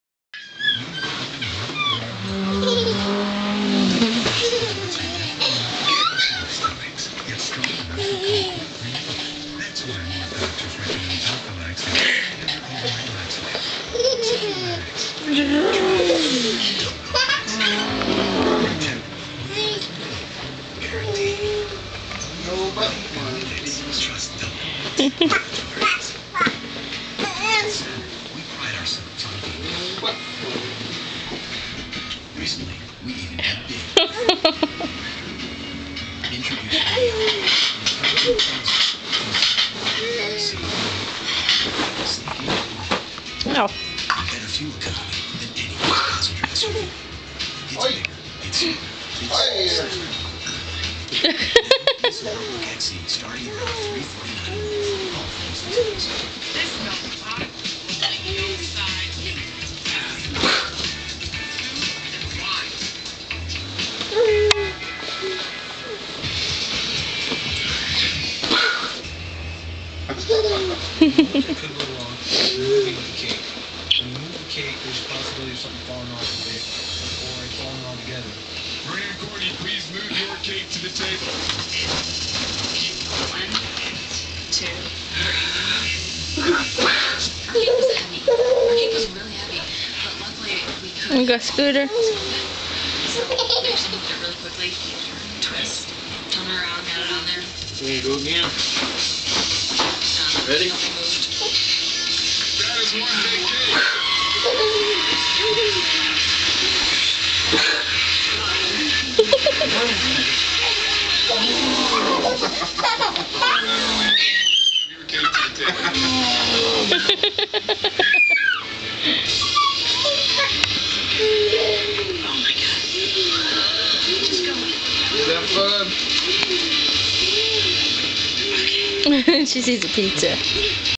Daddy swinging Maya in beanbag, 4-17-07
Jeremiah swinging Maya around in the beanbag chair.